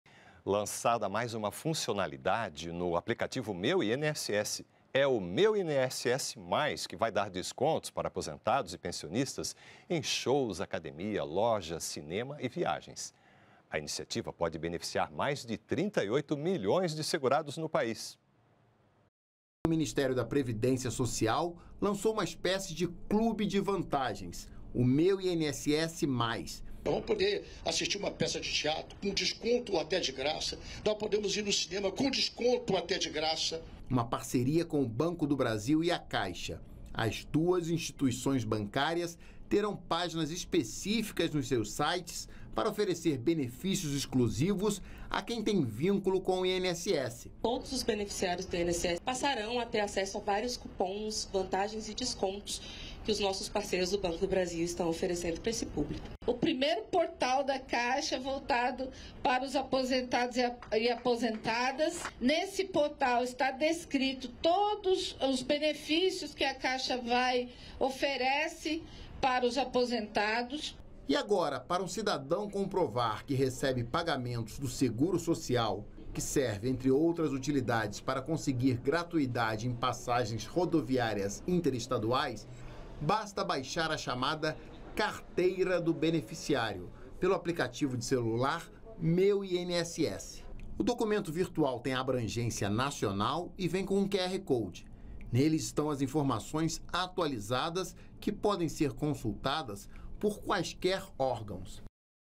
MEU INSS: APLICATIVO VIRTUAL TRAZ BENEFÍCIOS PARA APOSENTADOS E PENSIONISTAS
23.05.23 - Lançada mais uma funcionalidade no aplicativo Meu INSS.  É o Meu INSS Mais, que vai dar descontos para aposentados e pensionistas em shows, academias, lojas, cinemas e viagens.